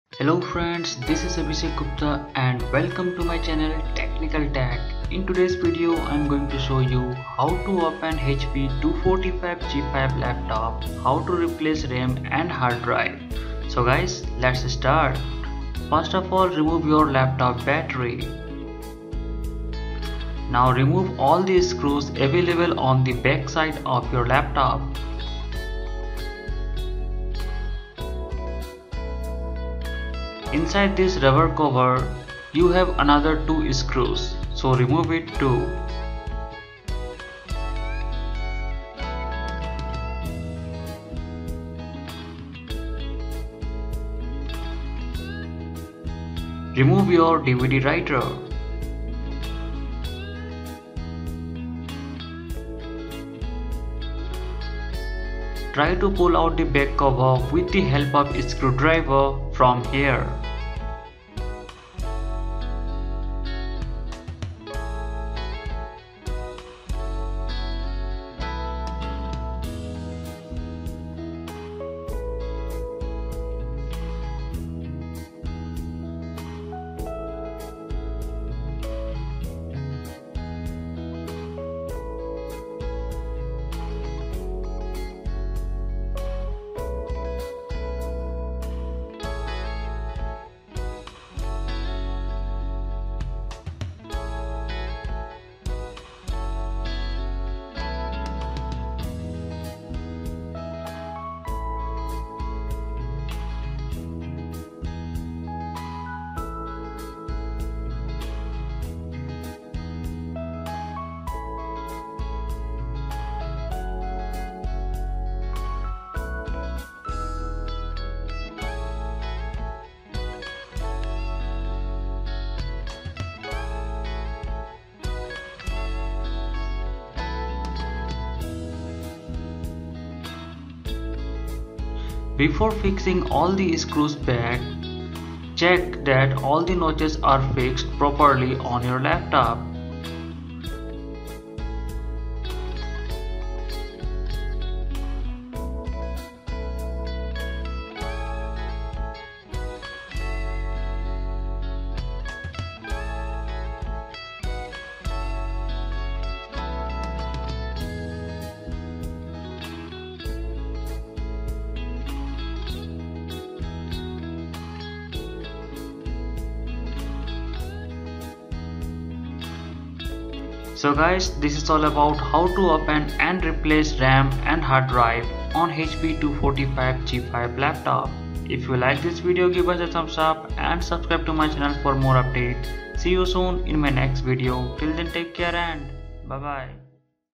How to open HP 245 G5 Laptop and replace RAM & HDD
How to open HP 245 G5, HP 245 G4, HP 240 G5, HP 250 G5 and replace RAM and HDD.